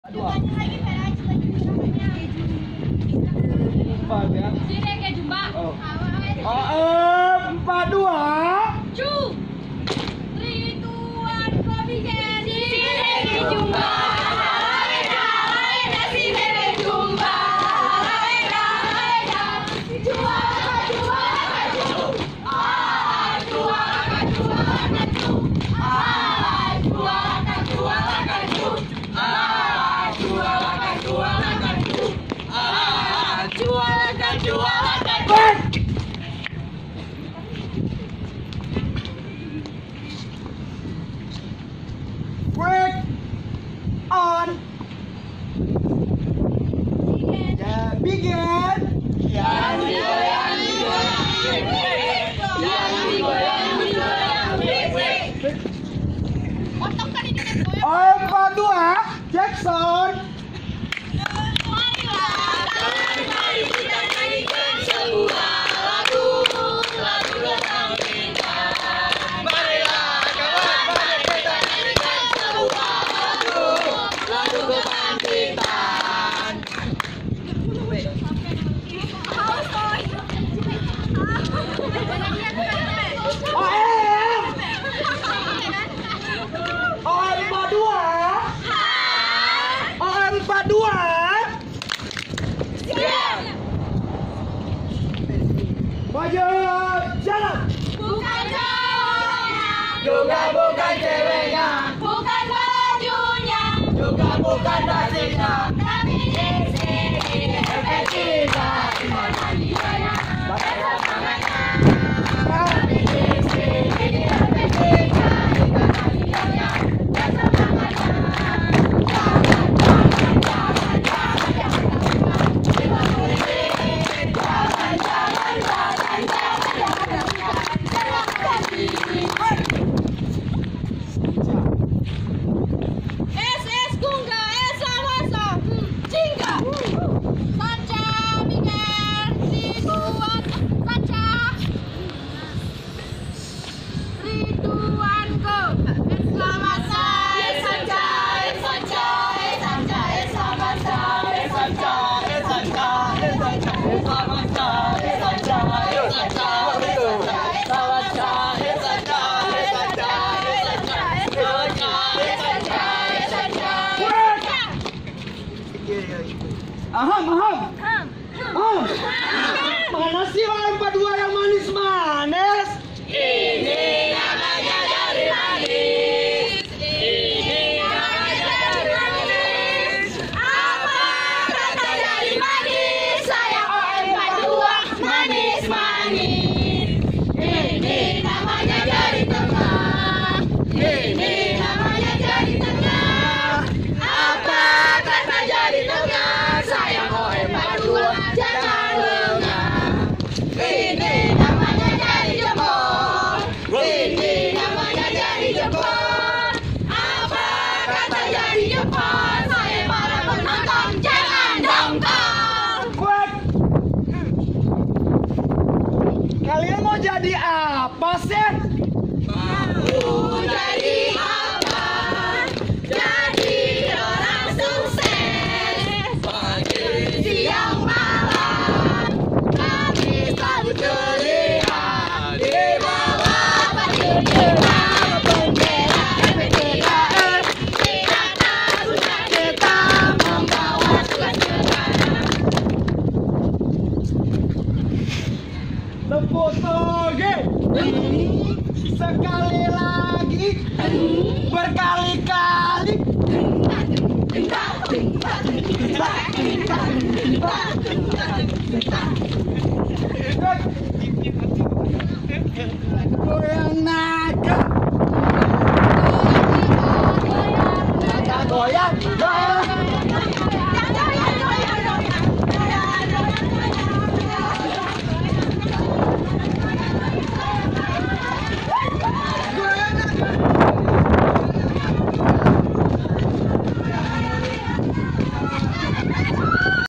YEL YEL MAHASISWA LP3I COLLEGE BANDA ACEH KELAS OM 42 TAHUN 2018
Video yel yel Mahasiswa LP3I College Banda Aceh Kelas OM 42 Tahun 2018 dalam rangka mengikuti Lomba Yel yel LP3I Colllege Banda Aceh Menyambut HUT LP3I yang ke 29.